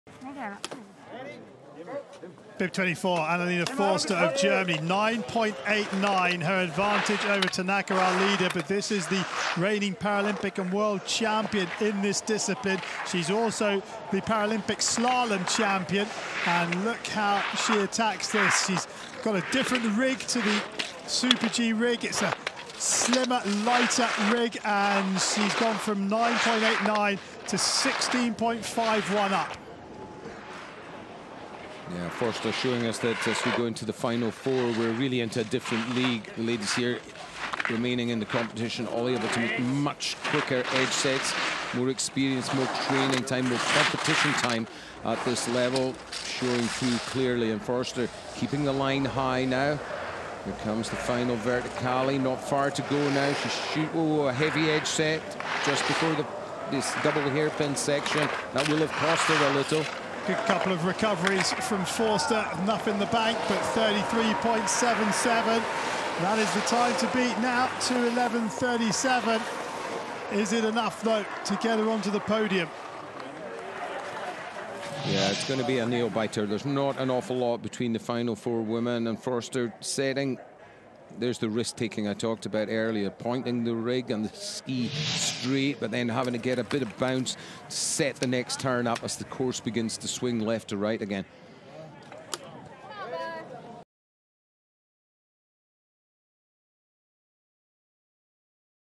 🇩🇪 Germany's Anna-Lena Forster claims GOLD at the Super Combined Slalom Sitting | Beijing 2022
Anna-Lena Forster of Team Germany claimed GOLD in the Para Alpine Skiing Women's Super Combined Slalom Sitting competition at Yanqing National Alpine Skiing Centre during day three of the Beijing 2022 Winter Paralympics.